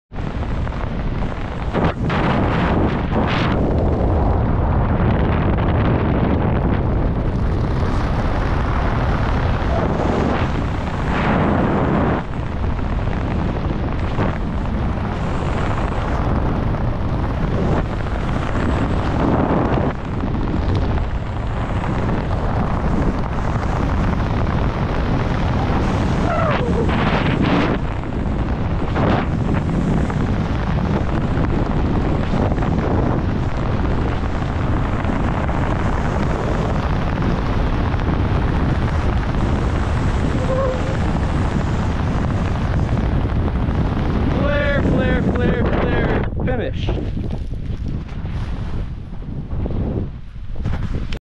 Skydive Santa Barbara Riverbed Landing Pattern
Casey Mongoven lands at Skydive Santa Barbara in September 2023.